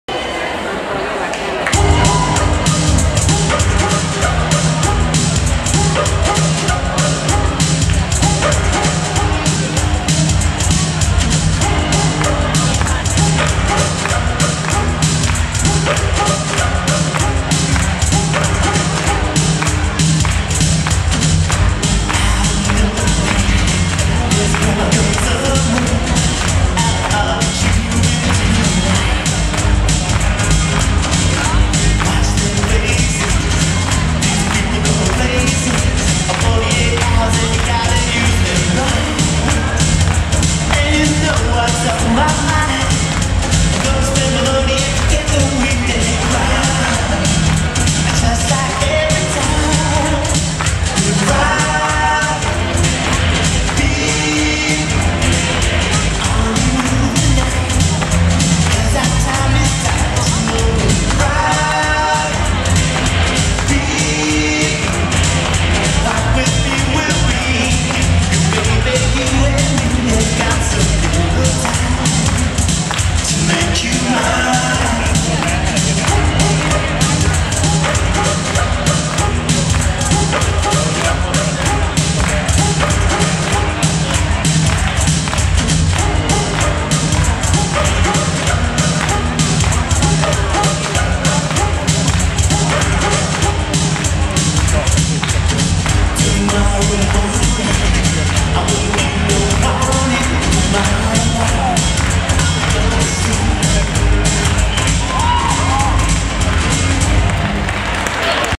Juhász Tamás - Mészáros Kata
Children és Serdülő Magyar Bajnokság, Pestszentimrei Sportkastély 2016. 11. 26.
Serdülő kategória: 24. hely